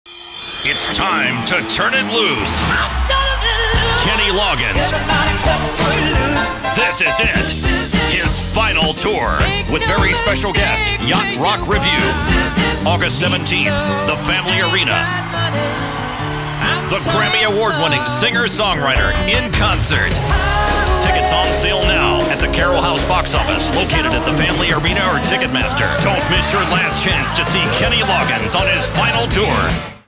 Kenny Loggins: His Final Tour at The Family Arena (August 17, 2023)
Celebrating a career that has seen him perform his ever-growing catalog of smash hit songs across the globe for nearly six decades Kenny Loggins brings his farewell tour to The Family Arena on August 17.